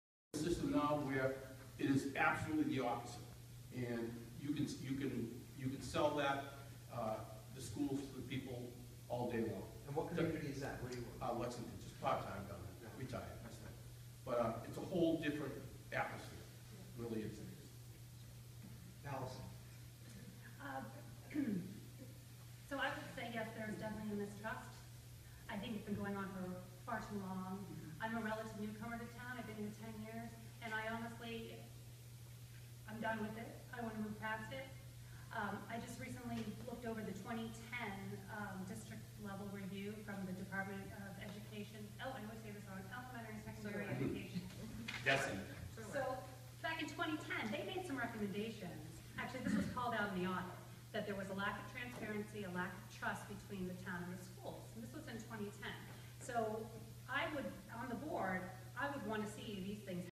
Tim Woods: it's better in Lexington. Allison Volpe agrees there's distrust of Dracut School Comm. @
Tim Woods: it's better in Lexington. Allison Volpe agrees there's distrust of Dracut School Comm. @lowellsunnews